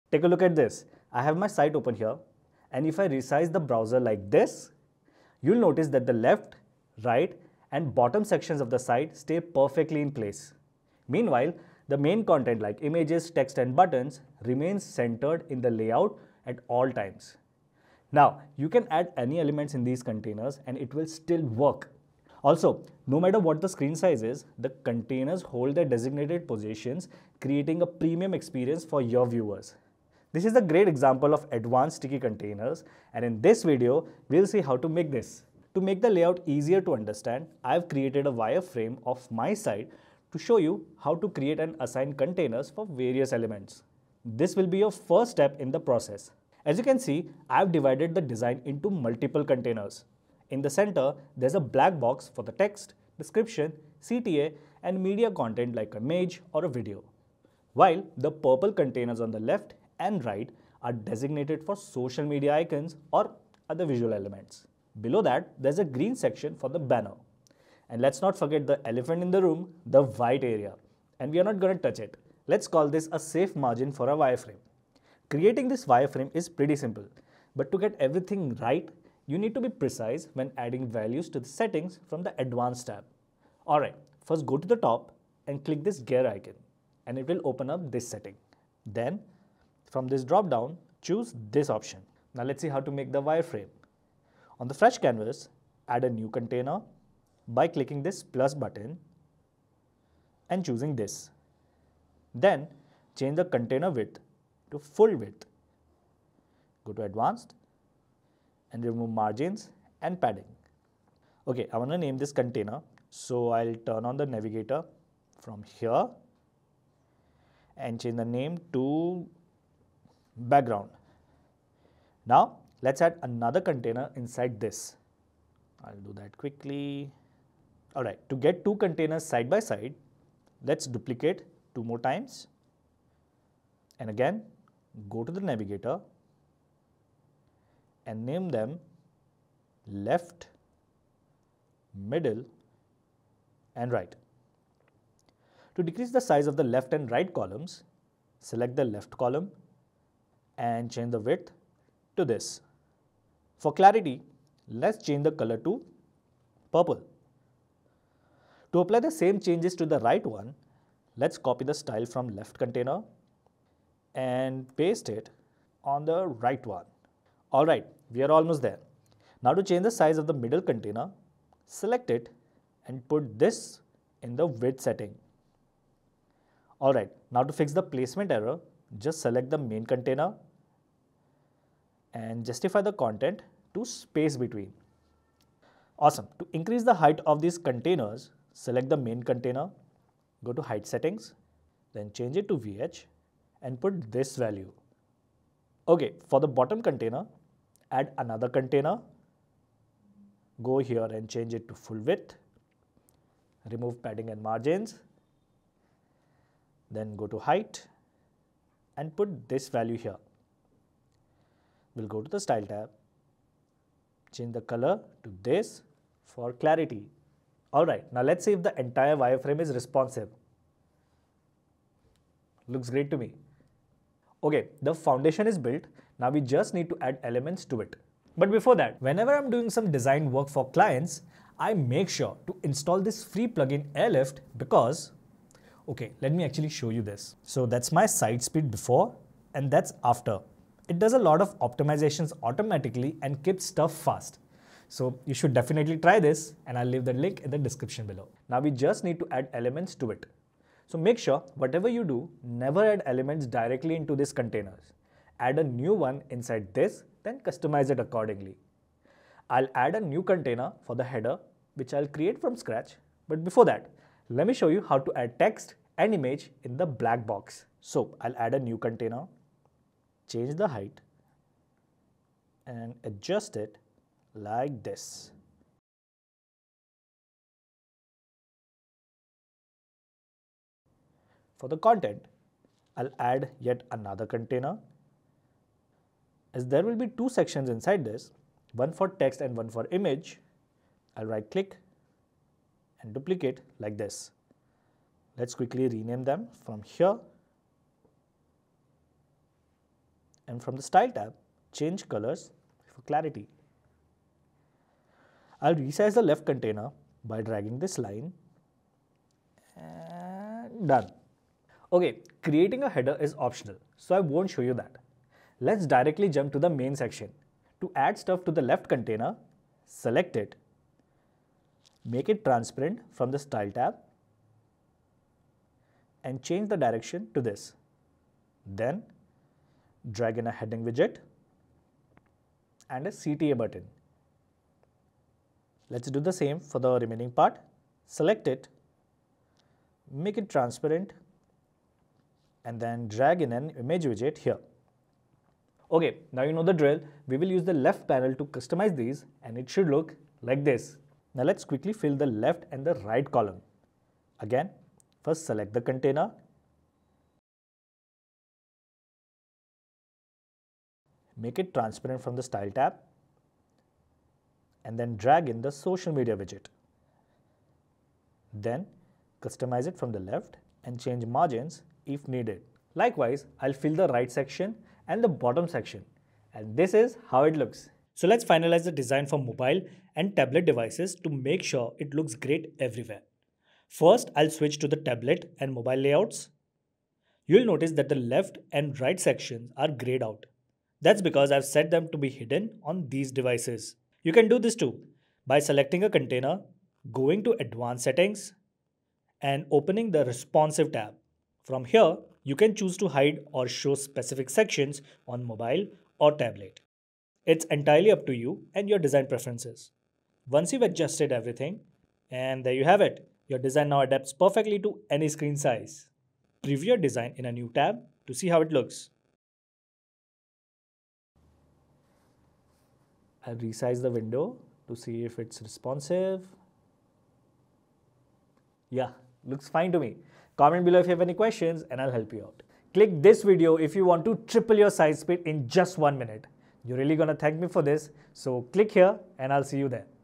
Elementor's New Responsive Full-Width Containers - Flexbox Tutorial
⭐ Giveaway: Our 100% FREE plugin makes any site 3X faster Want to create a seamless, responsive layout where key sections stay in place while your content remains perfectly centred? In this video, I’ll show you how to build an advanced sticky container layout that adapts beautifully to any screen size. We’ll start with a wireframe, set up the necessary containers, and adjust widths, heights, and margins to get everything just right. You’ll also learn how to optimize for mobile and tablet views, ensuring a premium experience across all devices. By the end, your site will maintain its structure effortlessly, no matter how the screen resizes.

00:00 Can I make my design responsive?
01:23 Create wireframe for responsiveness
04:09 Add your content to wireframe
06:23 Make the design mobile-responsive